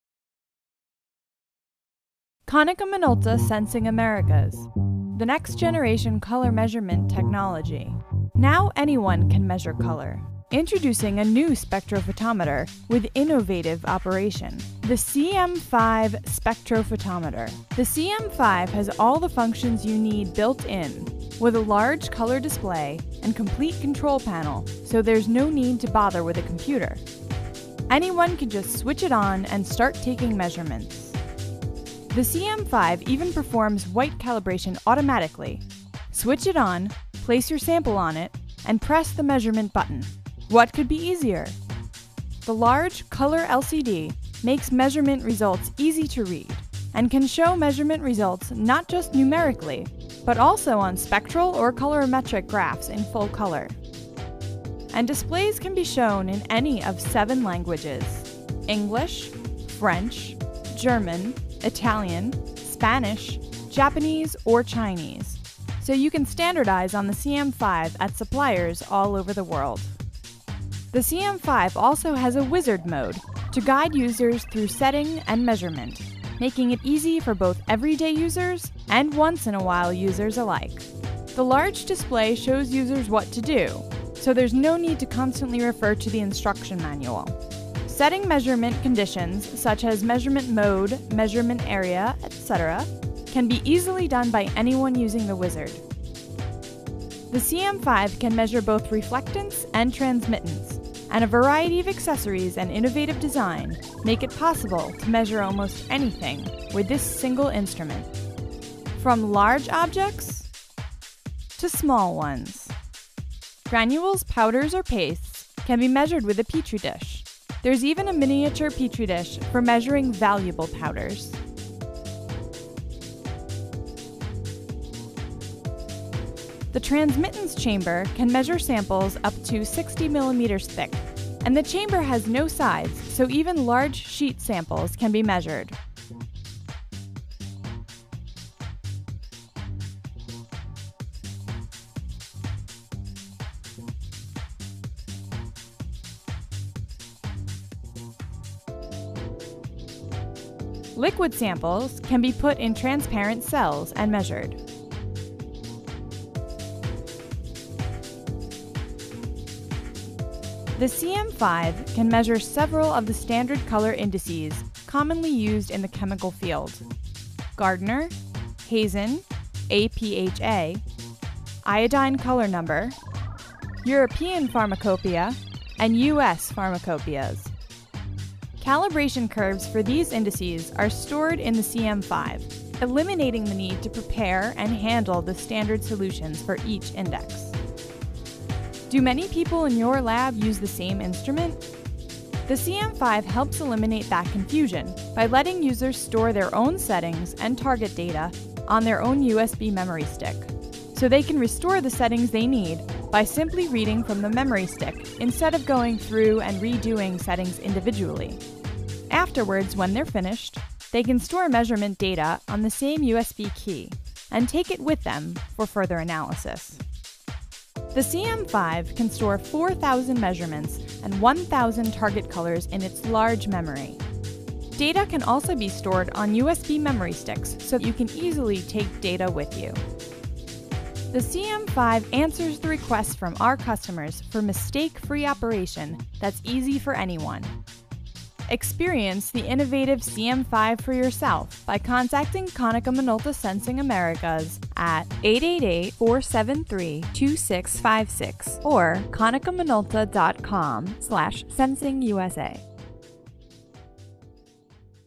CM-5 konica minolta
Công ty chúng tôi có bán máy đo màu CM-5 spectrophotometer của hãng Konica minolta Nhật Bản.
May do mau CM-5 konica minolta là dòng máy đo màu quang phổ đặt bàn, máy có thể đo vật rắn phẳng, đo màu bột , đo màu chất lỏng, đo màu dung dịch , máy đo màu vật trong suốt , bán trong suốt ,.
May do mau CM-5 là một trong những dòng máy đặt bàn có độ chính xác cao và rất thuận tiện khi đo màu sắc , Thao tác đo dễ dàng và nhanh chóng

May do so mau CM-5 spectrophotometer konic8quốc tế hiện nay về máy đo màu sắc

CM-5 konica minolta đo được các thông số như Lab, L*a*b*, Lch , XYZ, yxy, Gardner , apha, hazen , .
CM-5 Spectrophotometer

The CM-5 Spectrophotometer, a stand-alone, top-port color measuring instrument, is bench-top spectrophotometer with a large LCD screen and onboard software which eliminates the need for a separate computer. Color plots, as well as graphical and numerical data are displayed on the screen. The Operation Wizard, included with the unit, guides the user through the measurement process. Select Automatic Calibration to perform calibration every time the unit is turned on. Not only can the unit measure both transmittance and reflectance, but the user can can designate PASS/FAIL tolerances for each measurement as well.

It’s no problem to measure samples in many forms such as pellets, solids, gelatins, powders, and pastes with the CM-5 and it’s accessories, and that includes opaque, transparent, and translucent liquids. The CM-5 Spectrophotometer has the ability to take measurements for the chemical and pharmaceutical industries according to several standard indices: Gardner, Hazen/APHA, Iodine Color Number, US Pharmacopeia, and European Pharmacopoeia. The CM-5 can store and later recall settings for multiple-user situations,using USB memory sticks, or store data on USB memory sticks for computer analysis later. If a user need a more advanced analysis of data, the CM-5 Spectrophotometer can be connected to a PC with color software.
CM-5 Spectrophotometer

Easy 1-2-3 measurement – 1. Switch power on, 2. Position sample, 3. Press MEAS
Large color LCD – easy to read, displays data graphically and numerically, and shows PASS/FAIL judgments and color plots
Wizard for on-screen operation guidance – users making instrument settings are provided guidance by the Operation Wizard
Multi-user friendly – utilizing a USB memory stick, users can store each user’s settings and later read these settings back into the instrument for the next use. The USB can also be used to save target and measurement data
Automatic calibration – Set the unit to Automatic Calibration and every time the unit is turned on calibration can be performed automatically
8 languages- can be shown in English, German, Japanese, Italian, French, Portuguese, Spanish, and simplified Chinese
Top-port design – positioning the sample is easy with the top-port design
Three measuring areas – adapt to different sample sizes with available target masks, including 30mm, 8mm, 3mm
Measures variety of samples – optional Petri dish accessory enables measurement of pellets, granules, powders, chunky samples, pastes, and opaque liquids. Simply place solids on top. For small samples the 3mm target mask is suitable, and for tiny unique samples, custom mini Petri dishes are available
Large transmittance chamber – a large transmittance chamber with no sides is revealed by sliding open the unit, so even large sample sheets up to 60mm thick can be inserted and measured
Liquid measurements using cells – can measure liquids with optional cells, lengths 2mm, 10mm, and 20mm in order to measure different densities
Internal calibration for standard chemical/pharmaceutical indices – Gardner, Hazen/APHA, Iodine Color Number, US Pharmacopeia and European Pharmacopoeia can all be internally calibrated
Spectral absorbance measurement – a variety of industries can utilize new Graphical and numerical Absorbance displays